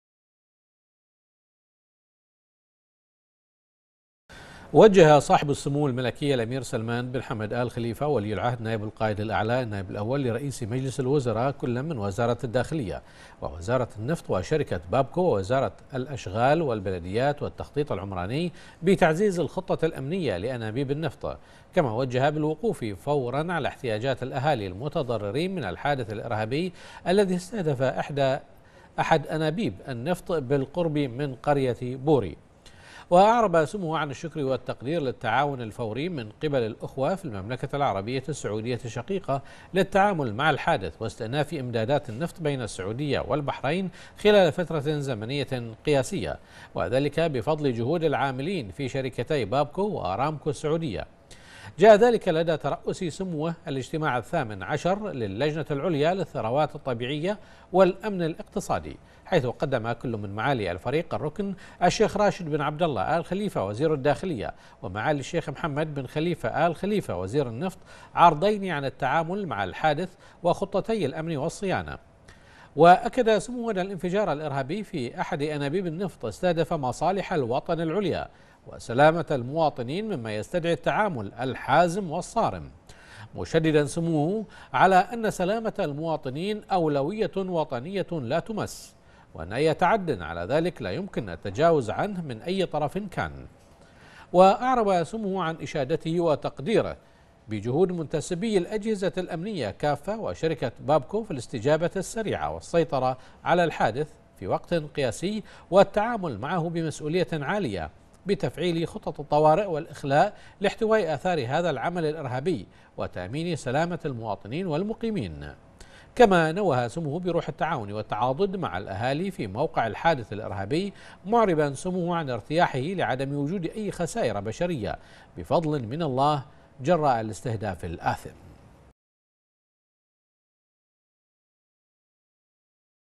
سمو ولي العهد يترأس إجتماع اللجنة العليا للثروات الطبيعية والأمن الاقتصادي
المنامة في 12 نوفمبر / بنا / وجه صاحب السمو الملكي الأمير سلمان بن حمد آل خليفة ولي العهد نائب القائد الأعلى النائب الأول لرئيس مجلس الوزراء كلًا من وزارة الداخلية ووزارة النفط وشركة "بابكو" ووزارة الأشغال والبلديات والتخطيط العمراني بتعزيز الخطة الأمنية لأنابيب النفط، كما وجه بالوقوف فورًا على احتياجات الأهالي المتضررين من الحادث الإرهابي الذي استهدف إحدى أنابيب النفط بالقرب من قرية بوري.